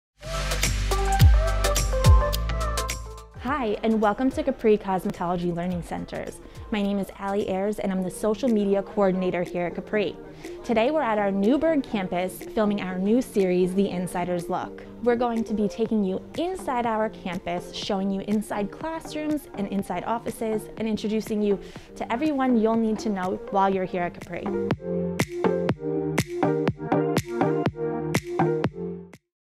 Insiders Look Newburgh Intro
Join us for our new series The Insiders Look.
We introduce you to Coaches, Staff and current students!